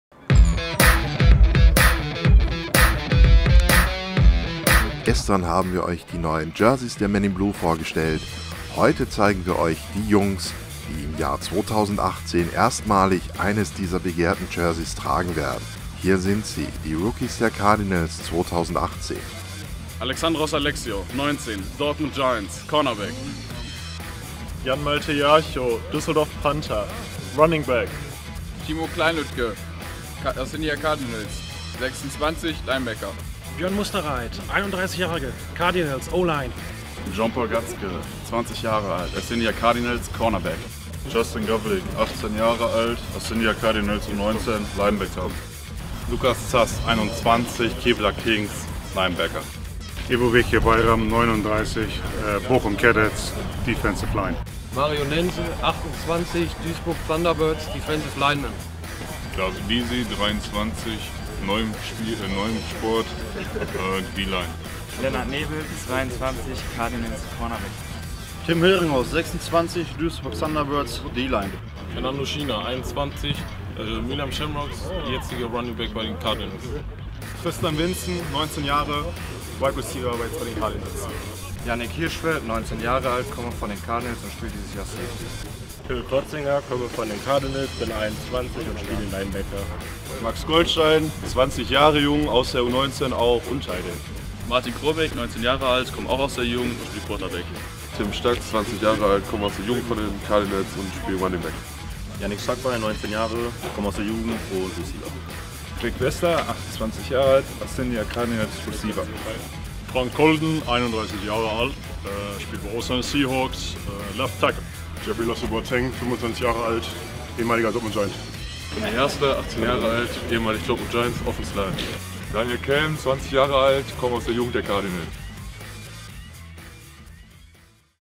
Assindia Cardinals Rookies 2018 - Cardinals Inside
Neue Saison, neue Gesichter. Auch in diesem Jahr werden viele neue Spieler das neue Jersey der MeninBlue tragen - ob aus der eigenen Jugend, als Vereinswechsler oder als Neueinsteiger.
Wir stellen vor: die neuen Cardinals!